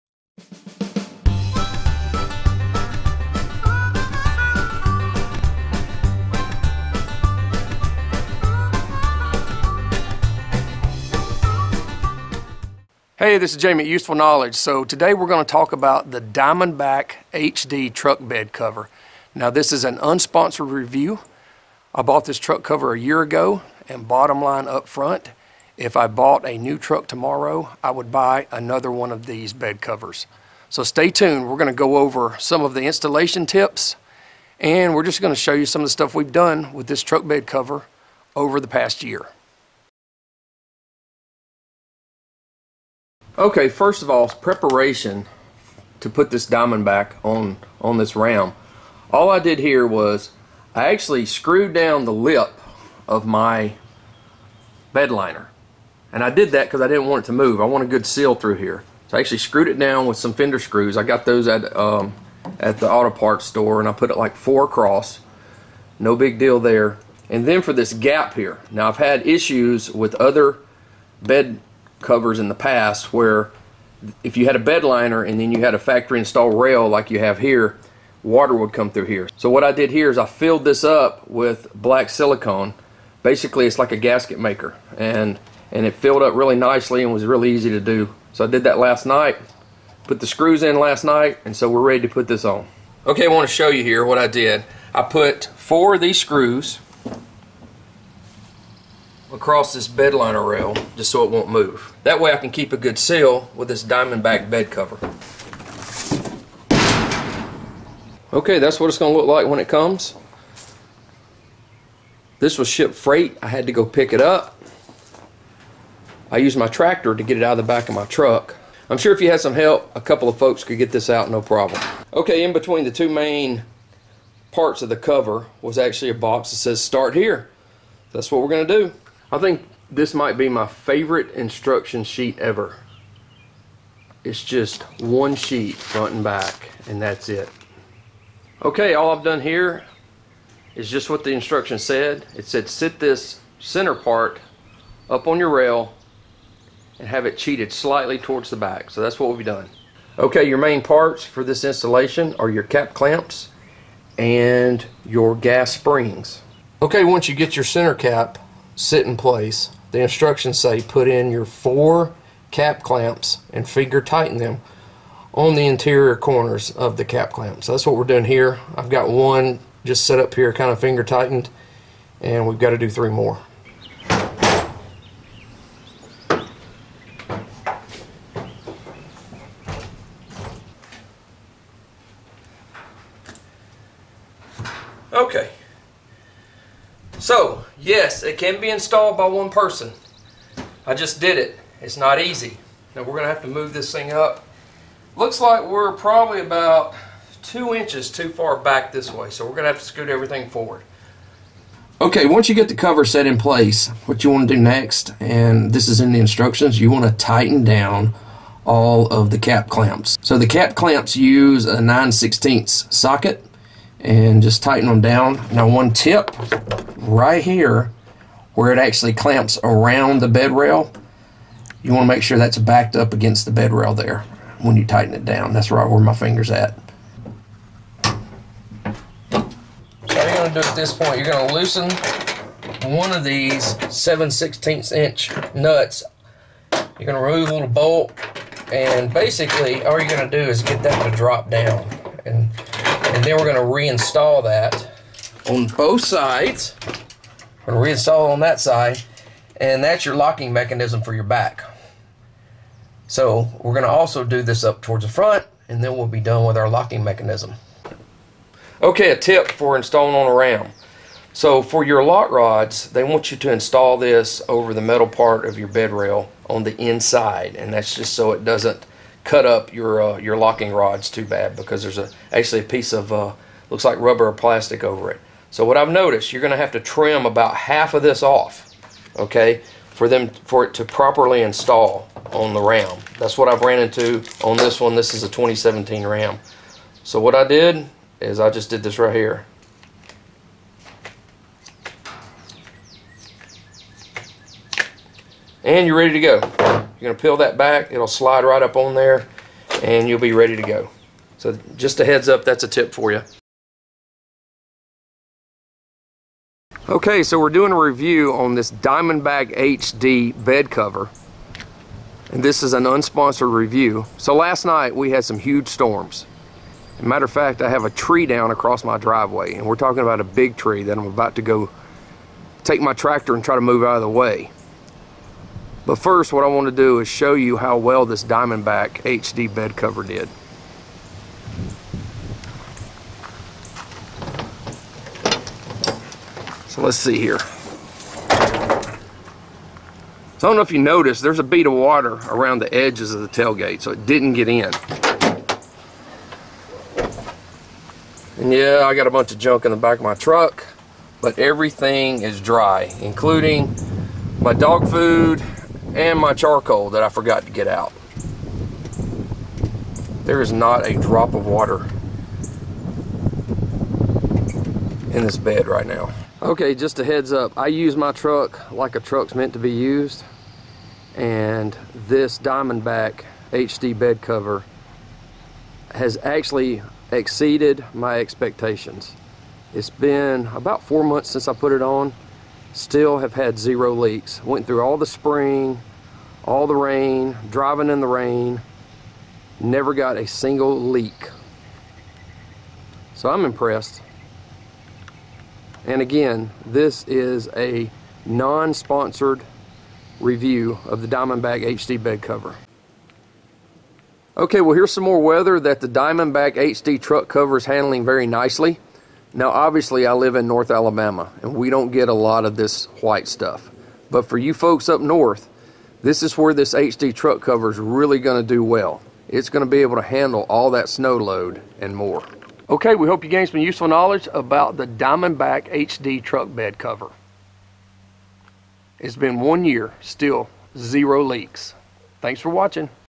Diamondback HD Truck Cover | Useful Knowledge
The Diamondback HD truck cover is a great product from Diamondback truck covers.  The Diamondback HD truck cover is made from aluminum and has a 1600-lb hauling capacity.  We installed this Diamondback truck cover on Jamie's 2017 Ram 2500.  After installation, we let Jamie use his truck as always.  He tows tractors, ATV's, feed corn, and anything he can fit on his trailers.

This Diamondback truck bed cover is truly heavy duty.  We performed an unsponsored review for a complete year.  We live in north Alabama.  That means all kinds of weather including extreme heat, high humidity, severe storms, sleet, snow, and lots of rain.

The neat thing about the Diamondback HD truck cover is the ability of walk or sit on it.  You can also haul anything on it.  Jamie even hauled a refrigerator on the one in the video.

Man with the Knowledge: Jamie Hardy
Editor: John Andrews (UberonN)
Creator: Mark Davies

The Diamondback HD truck cover is a product this definitely worth every penny that it costs.